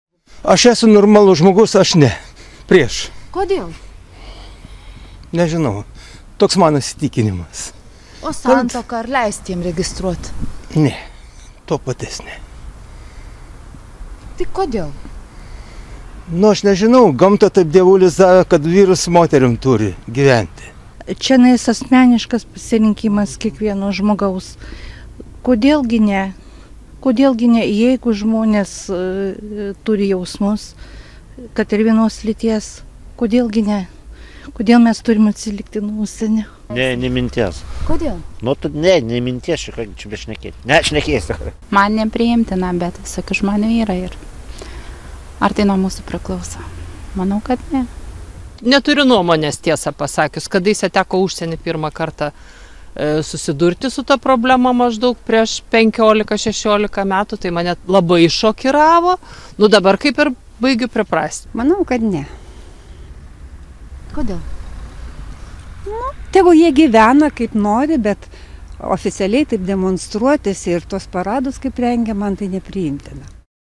Lietuvoje daugėja pritariančių homoseksualų partnerystei
Homoseksualų partnerystei pritartų 11 proc., o vienos lyties asmenų santuokai tik 7 proc. Lietuvos gyventojų. Tai rodo naujausia „Vilmorus“ atlikta apklausa. Gyventojų nuomonės tyrimą užsakę Lietuvos notarų rūmai informuoja, kad prieš siūlymus, kad būtų galima registruoti tos pačios lyties asmenų partnerystę, pasisako 74 proc. apklaustųjų. O siūlymams, kad būtų galima užregistruoti tos pačios lyties asmenų santuoką, prieštarauja 80 proc. respondentų. Palyginti su analogiškomis 2013 metų apklausomis, neigiamai vertinančių vienalytes partnerystes sumažėjo, o pritariančiųjų vienos lyties asmenų partnerystei skaičius paaugo.